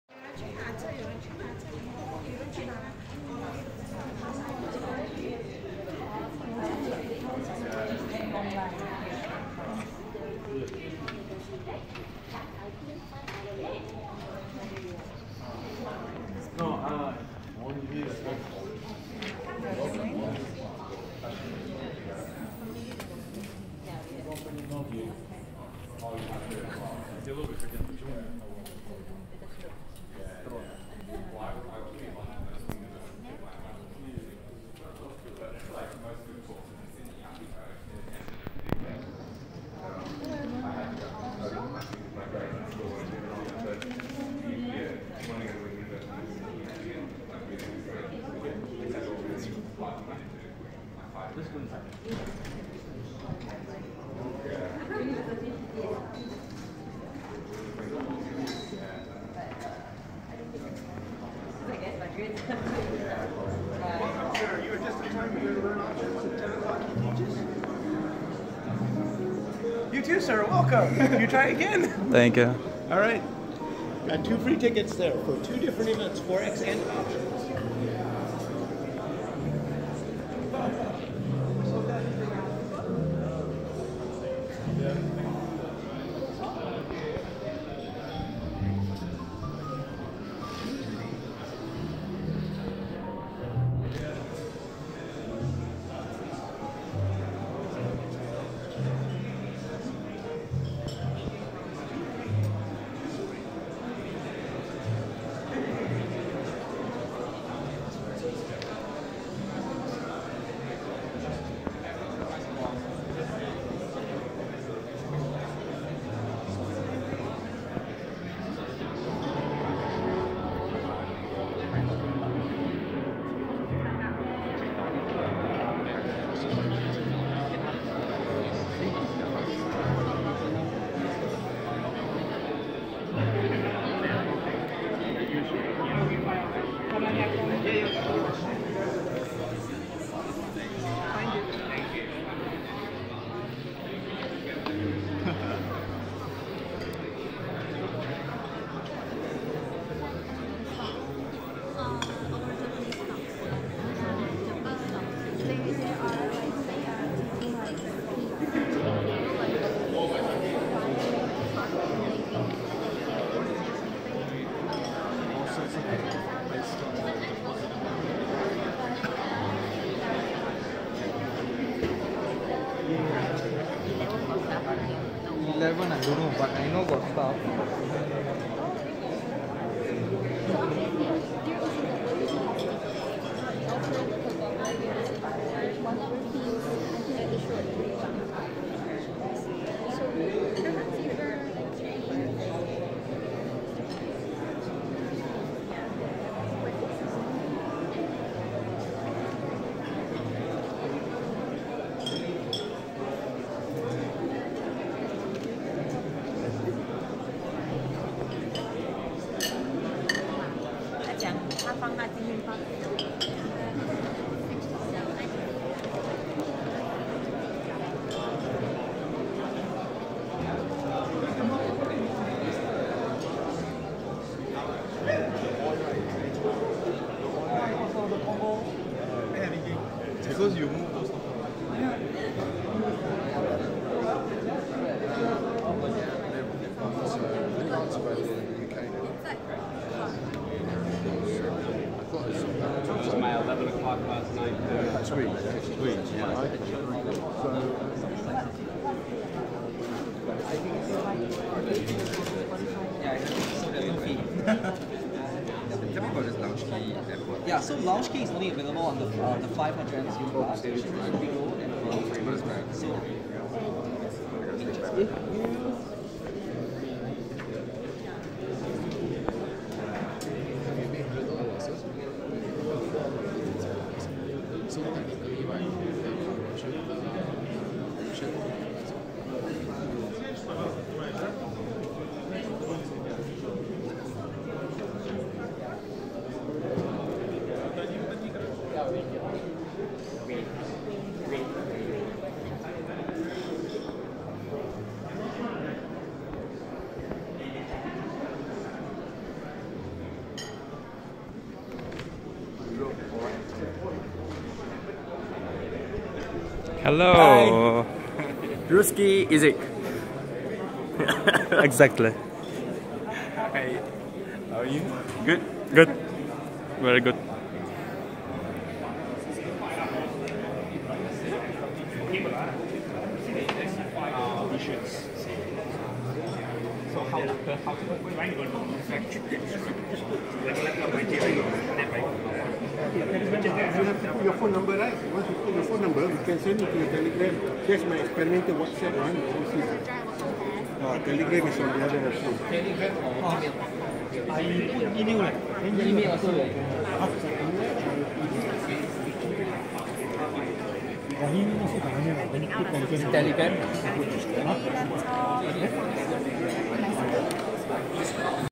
Welcome! Traders Fair & Crypto Expo - Singapore 2019
We have started👏 Welcome🤗🤗
Traders Fair & Crypto Expo - Singapore 2019
9:30 – 18:00
🏬 Marina Bay Sands Convention Centre, 3rd L Heliconia
The entrance is free, just register: